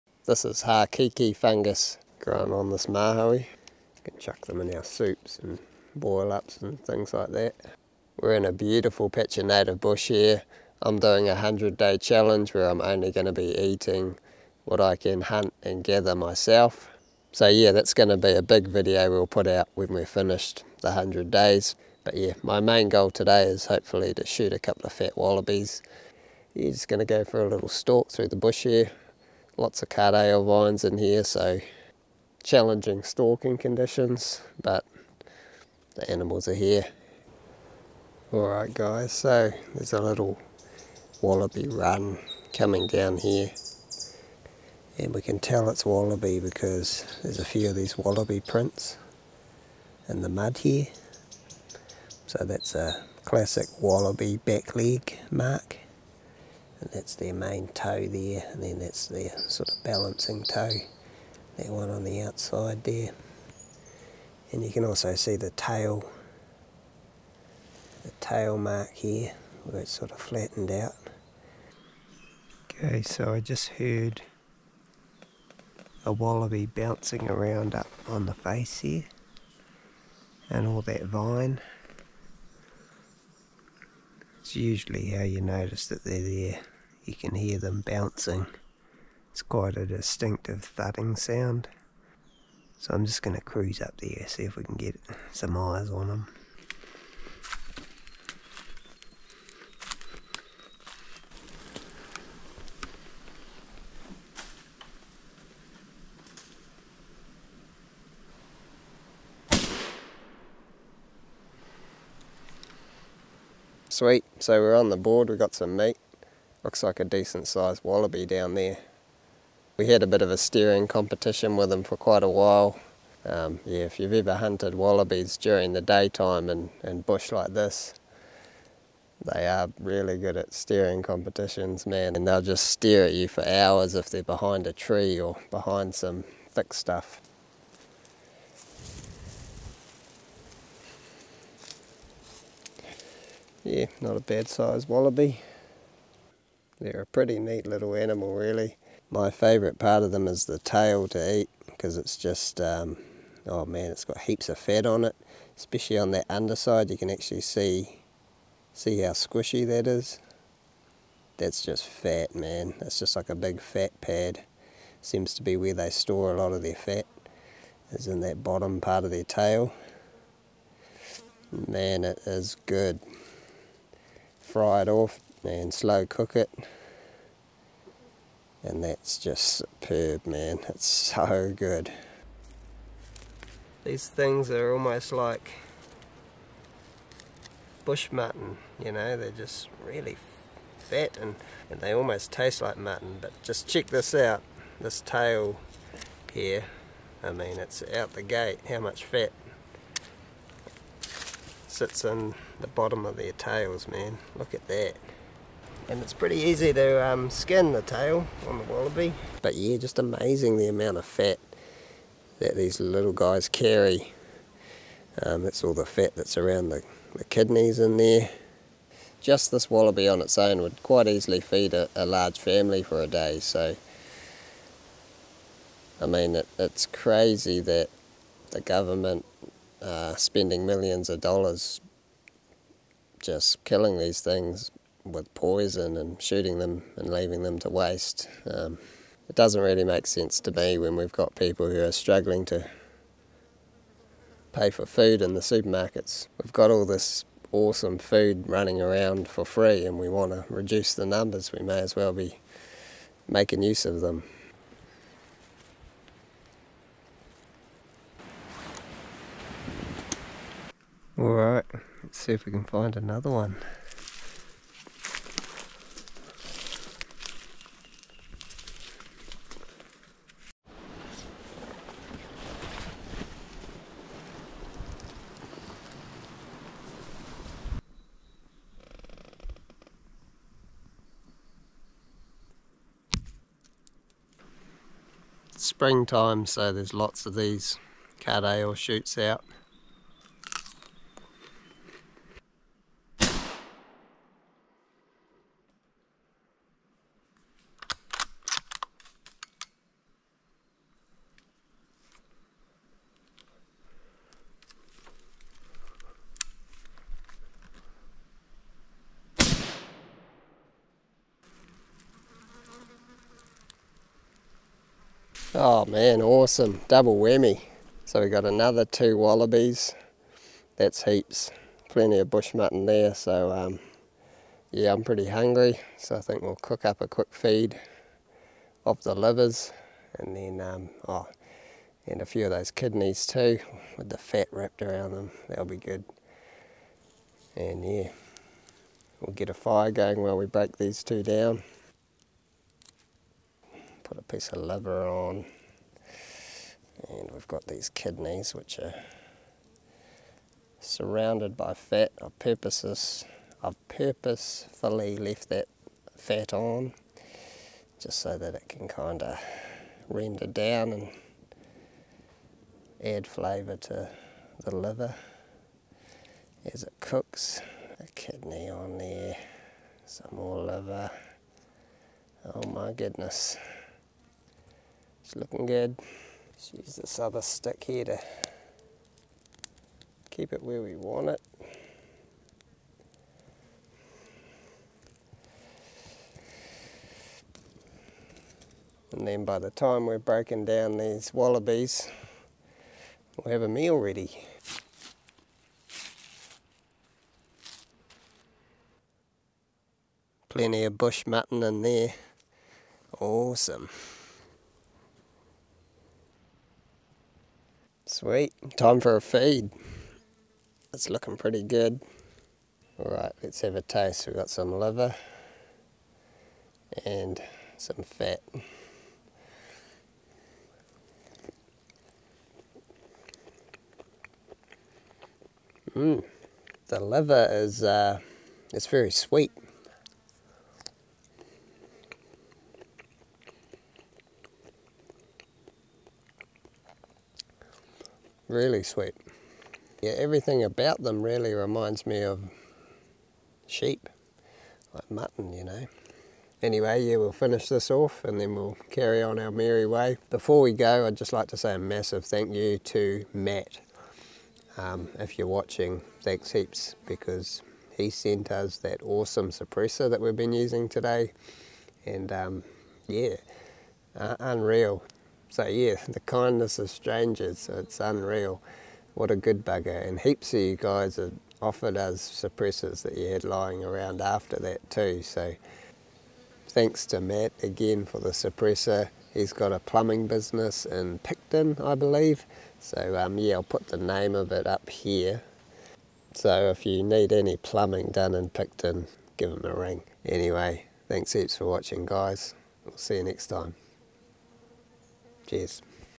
Wallaby Catch, Forage, COOK (60 Day Hunt & Gather Challenge)
Harvesting some Wallabies in the bush to keep us going. Today is day 4 of our 60 day challenge